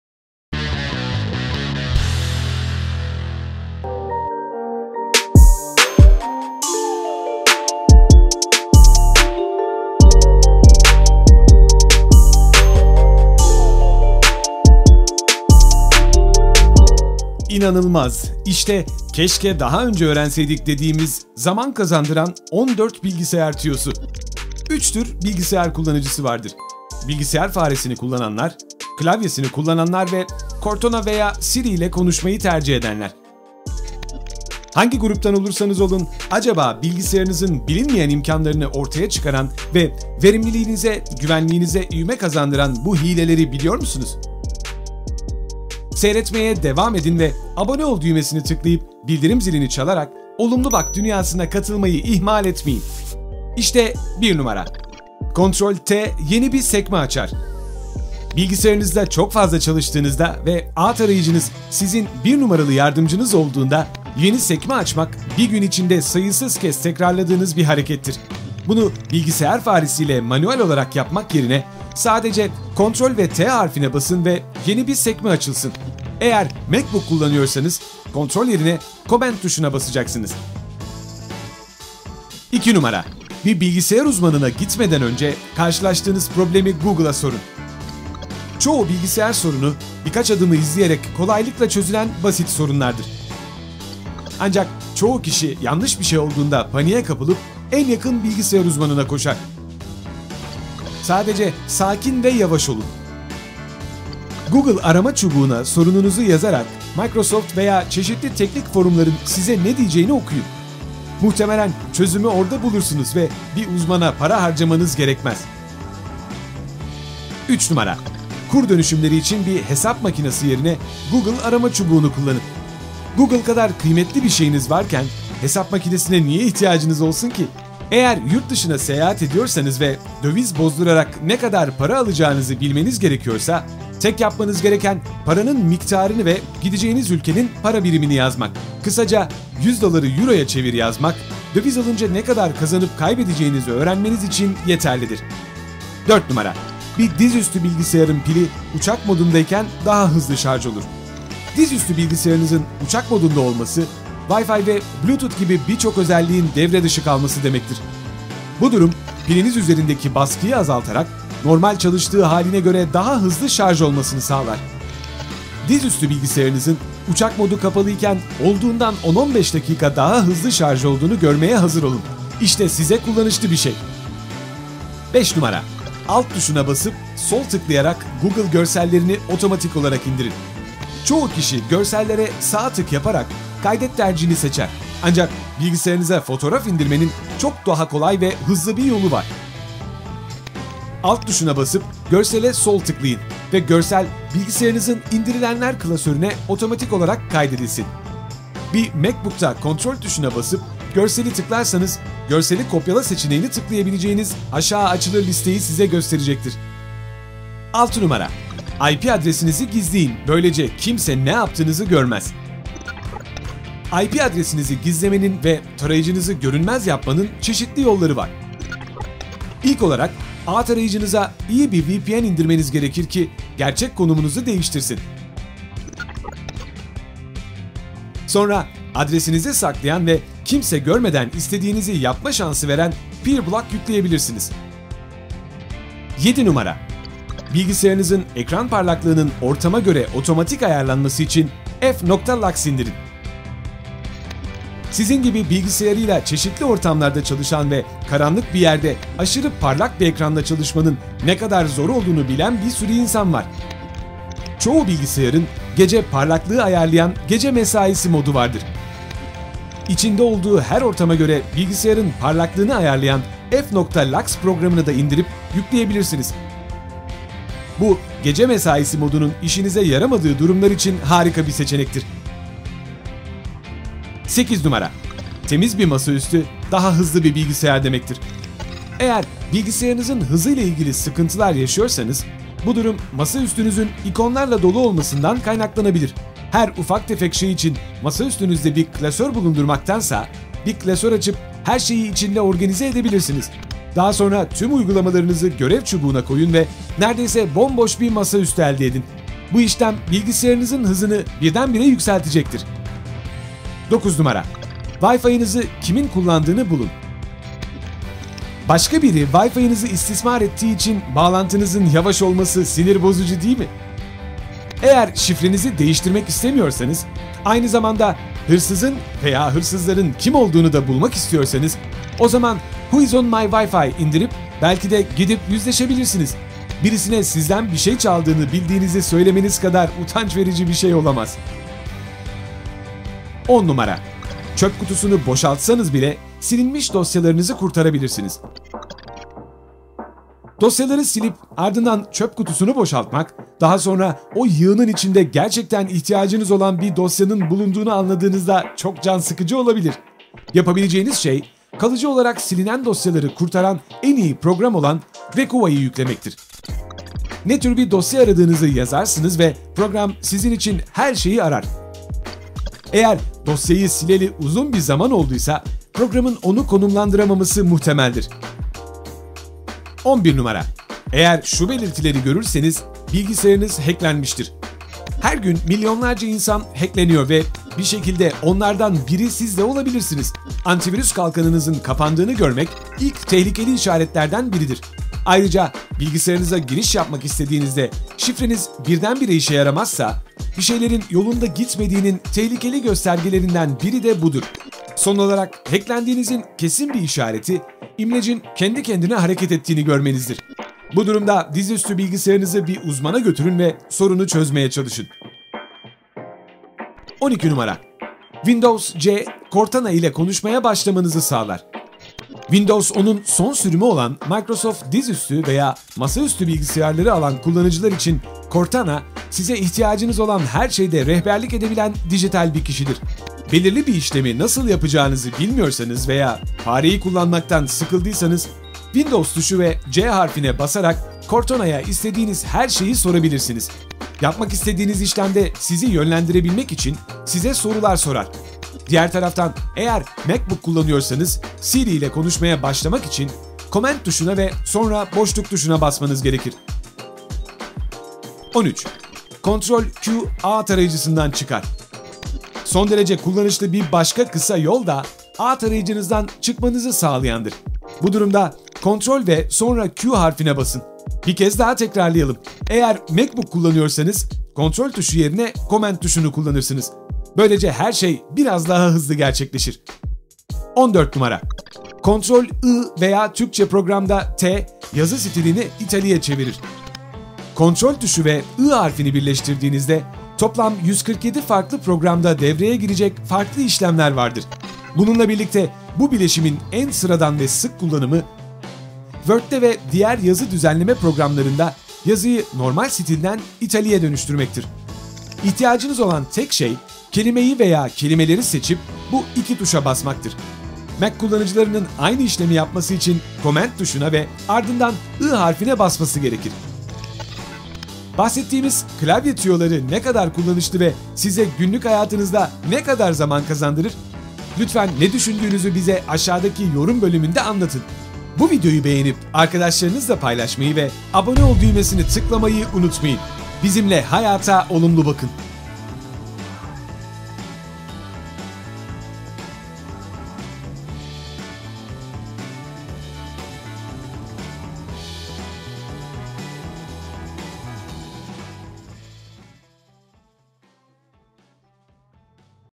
Keşke Daha Önce Öğrenseydik Dediğimiz Zaman Kazandıran 14 Bilgisayar Tüyosu
3 tür bilgisayar kullanıcısı vardır: bilgisayar faresini kullananlar, klavyesini kullananlar ve Cortona veya Siri ile konuşmayı tercih edenler. Hangi kategoriye girerseniz girin, bilgisayarınızın her zaman sahip olduğu bilinmeyen imkânlarını ortaya koyan belirli kısa yolların ve tüyoların kullanışlılığını inkâr edemezsiniz. Bu akıllıca yöntemler günlük bilgisayar deneyiminizi geliştirir ve verimliliğiniDSVCze, güvenliğinize ivme kazandırır.
Bilgisayarınızı  eğlence veya iş için kullanırken size fazlasıyla yardımcı olacak 14 bilgisayar tüyosunu derledik.

ZAMAN BİLGİLERİ:
1. Ctrl + T yeni bir sekme açar 0:54
2. Bir bilgisayar uzmanına gitmeden önce, karşılaştığınız problemi Google’a sorun 1:24
3. Kur dönüşümleri için bir hesap makinesi yerine Google arama çubuğunu kullanın 1:57
4. Bir dizüstü bilgisayarın pili uçak modundayken daha hızlı şarj olur 2:27
5. Alt tuşuna basıp sol tıklayarak Google görsellerini otomatik olarak indirin 2:57
6. IP adresinizi gizleyin, böylece kimse ne yaptığınızı görmez 3:29
7. Bilgisayarınızın ekran parlaklığının ortama göre otomatik ayarlanması için “f.lux” indirin 3:57
8. Temiz bir masaüstü, daha hızlı bir bilgisayar demektir 4:35
9. Wi-Fi’ nızı kimin kullandığını bulun 5:04
10. Çöp kutusunu boşaltsanız bile, silinmiş dosyalarınızı kurtarabilirsiniz 5:37
11. Eğer bu belirtileri görürseniz, bilgisayarınız heklenmiştir 6:14
12. Windows + C, Cortana ile konuşmaya başlamanızı sağlar 6:49
13. Ctrl+Q ağ tarayıcısından çıkar 7:30
14. Ctrl + I yazı stilini italiğe çevirir 7:55

ÖZET:
- Yeni sekme açmak bir gün içerisinde sayısız kez tekrarladığınız bir harekettir. Bunu bilgisayar faresiyle manuel olarak yapmak yerine, sadece “ctrl” ve T harfine basın.
- Google arama çubuğuna sorununuzu yazarak, Microsoft veya çeşitli teknik forumların size ne diyeceğini okuyun.
- Eğer yurt dışına seyahat ediyorsanız ve döviz bozdurarak ne kadar para alacağınızı bilmeniz gerekiyorsa, tek yapmanız gereken paranın miktarını ve gideceğiniz ülkenin para birimini yazmak.
- Dizüstü bilgisayarınızın uçak modunda olması, Wi-Fi ve Bluetooth gibi birçok özelliğin devre dışı kalması demektir. Bu durum, piliniz üzerindeki baskıyı azaltarak, normal çalıştığı haline göre daha hızlı şarj olmasını sağlar.
- “Alt” tuşuna basıp, görsele sol tıklayın ve görsel, bilgisayarınızın İndirilenler klasörüne otomatik olarak kaydedilsin.
- IP adresinizi gizlemenin ve tarayıcınızı görünmez yapmanın çeşitli yolları var. İlk olarak, ağ tarayıcınıza iyi bir VPN indirmeniz gerekir ki, gerçek konumunuzu değiştirsin.
- Çoğu bilgisayarın, gece parlaklığı ayarlayan gece mesaisi modu vardır.
- Eğer bilgisayarınızın hızıyla ilgili sıkıntılar yaşıyorsanız, bu durum masaüstünüzün ikonlarla dolu olmasından kaynaklanabilir. En ufak tefek şey için masaüstünüzde bir klasör bulundurmaktansa, bir klasör açıp, her şeyi içinde organize edebilirsiniz.
- Eğer şifrenizi değiştirmek istemiyorsanız, aynı zamanda hırsızın (veya hırsızların) kim olduğunu da bulmak istiyorsanız, o zaman Who Is on My Wi-Fi indirip, belki de gidip yüzleşebilirsiniz.
- Dosyaları silip, ardından çöp kutusunu boşaltmak, daha sonra o yığının içinde gerçekten ihtiyacınız olan bir dosyanın bulunduğunu anladığınızda çok can sıkıcı olabilir. Yapabileceğiniz şey, kalıcı olarak silinen dosyaları kurtaran en iyi program olan Recuva yüklemektir.
- Her gün milyonlarca insan hackleniyor ve bir şekilde onlardan biri siz de olabilirsiniz. Antivirüs kalkanınızın kapandığını görmek tehlikeli ilk işaretlerden biridir.
- Son derece kullanışlı bir başka kısa yol da, ağ tarayıcınızdan çıkmanızı sağlayandır. Bu durumda, “ctrl” ve sonra Q harfine basın.
-“Ctrl” tuşu ve I harfini birleştirdiğinizde, toplam 147 farklı programda devreye girecek farklı işlemler vardır. Bununla birlikte, bu bileşimin en sıradan ve sık kullanımı, Word’de ve diğer yazı düzenleme programlarında yazıyı normal stilden italiğe dönüştürmektir.